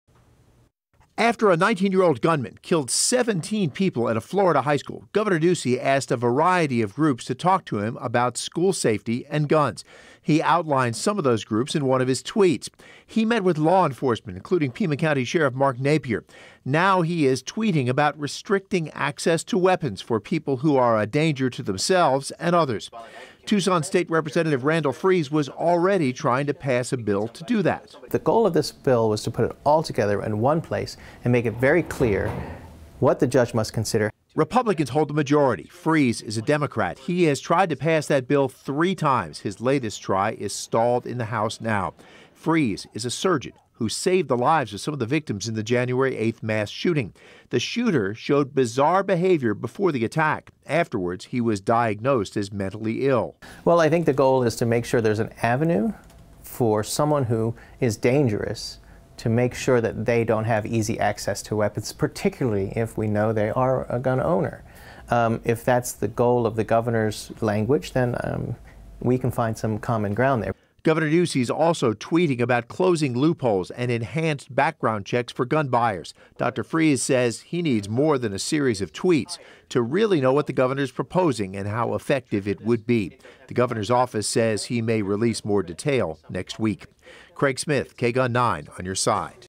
Gov Ducey may back laws to seize guns from dangerous people
His tweets talk about gun changes